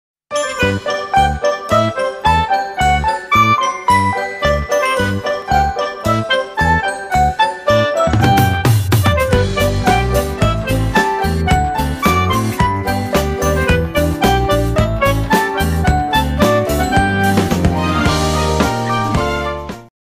Cindy is paving mosaic chip in fogas mosaic workshop
Cindy is paving mosaic chip in fogas mosaic workshop. Paving a sheet of mosaic tile is not easy work.